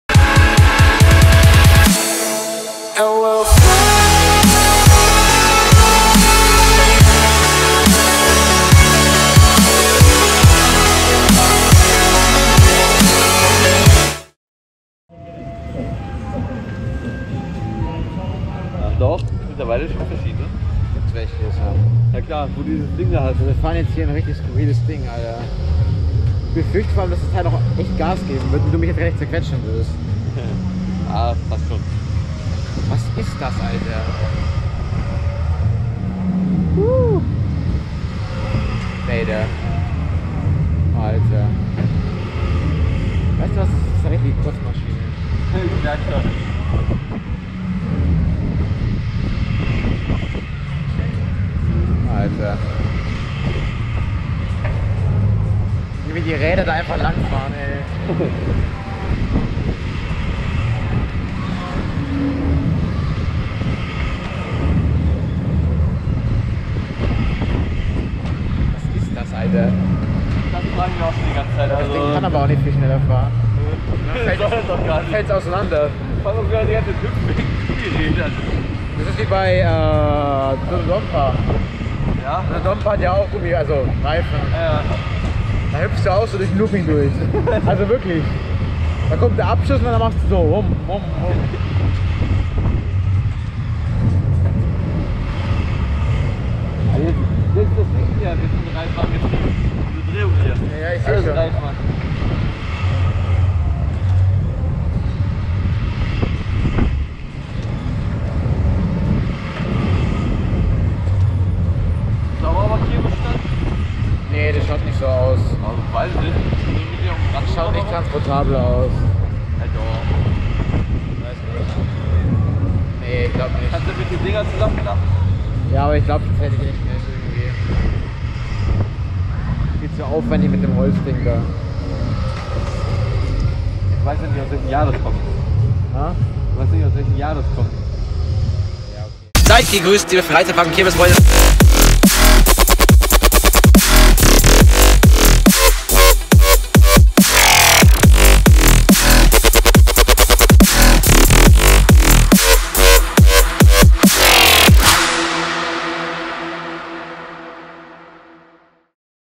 Twister - onride/POV | Southport Pleasureland England 2022
🎡🎢: Twister (Emmo Kreekel Calypso)

🗺: Southport, England 🏴󠁧󠁢󠁥󠁮󠁧󠁿

🔭: Cullmann 55460 MUNDO 525M
          Endcard:
                    TKC Endcard - Epidemix

Travel, Themeparks and Funfairs: TheKirmesCrazy